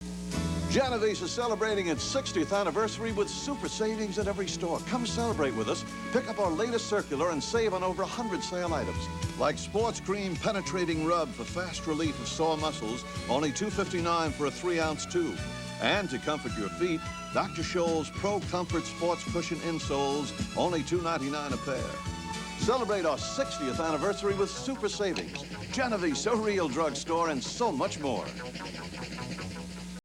1984 - Genovese Drug Stores
Local New York ad for the old Genovese pharmacy chain.